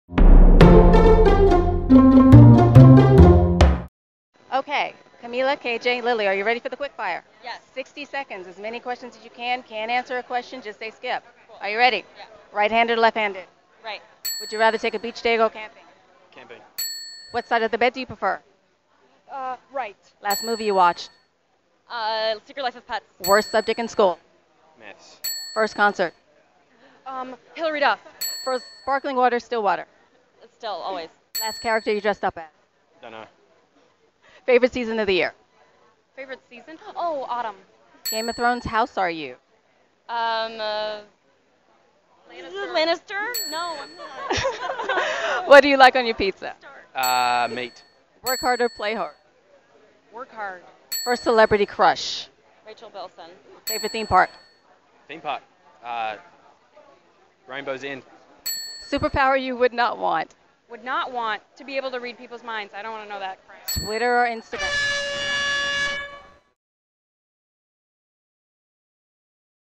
60 Seconds with Riverdale's Camila Mendes, KJ Apa, Lili Reinhart
Welcome to 60 Seconds With... where you get to know your favorite actors/musicians and the points don't matter! Without further ado, please welcome, a big part of the Riverdale cast: Camila Mendes, KJ Apa, Lili Reinhart.

Want more videos like this one? We need your help